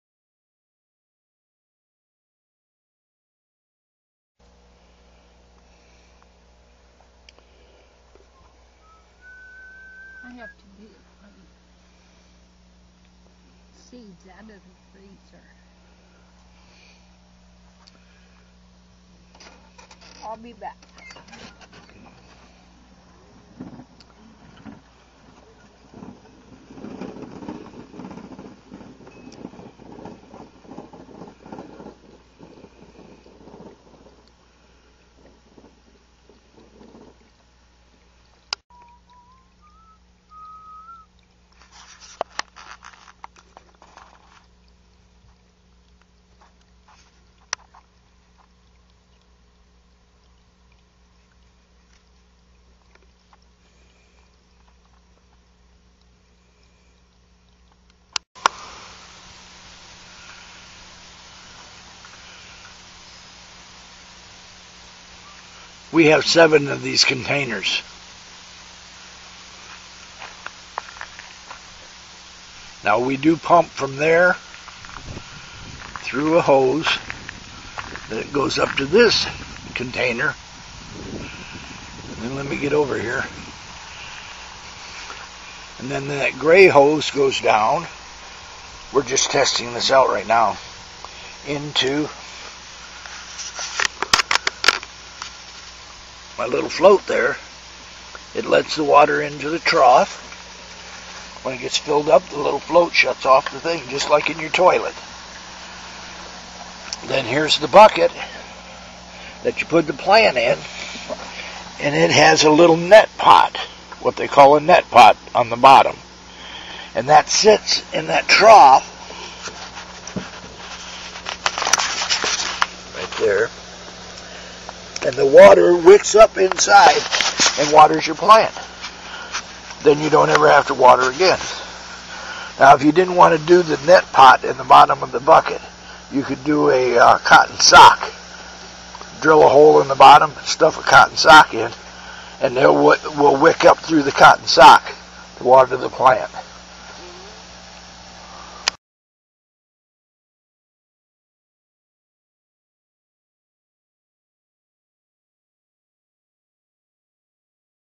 John Grab from Mulberry Arkansas builds his first Self Watering Rain Gutter Grow System!
How To Build A Self Watering Rain Gutter Grow System!
Ok so you want to build a Rain Gutter Grow System! Here is what you will need! 2 10 ft treated 2x4's 1 10 ft rain gutter and 2 end caps.I prefer the U snap brand because you can use standard pvc cement to glue on the ends. You will need 3 inch net cups they are cheap! Drill a 2/78 inch hole in your container that you are going to plant in and the 3 inch net cup fits perfectly. Also you'll need some pole barn roofing screws because they have a water tight seal on them. also a mini float valve to regulate water I use the one with the male 1/2 inch threads and buy a 1/2 threaded garden hose adapter to connect to it. You also can get the 3/8 barbed version and some 3/8 plastic hose and daisy chain a bunch together.You can use any good potting mix to grow your veggies in Not potting soil! Miracle grow potting mix works great or you can mix your own by using 2/3rds peat moss and 1/3 well rotted manure or compost and a couple of good handfuls of perlite! It lightens the soil. I also add about a cup of garden lime or dolomite, I also throw in a handful of Epsom salts. A standard batch I will make would be 2 5 gallon buckets of peat 1 bucket of compost or well rotted manure 1 cup garden lime and a handful Epsom salts. It has worked great for me Here is a link to the video on how I built the unit! Good Luck and let me know how you like the system! If you have any question please message me! Larry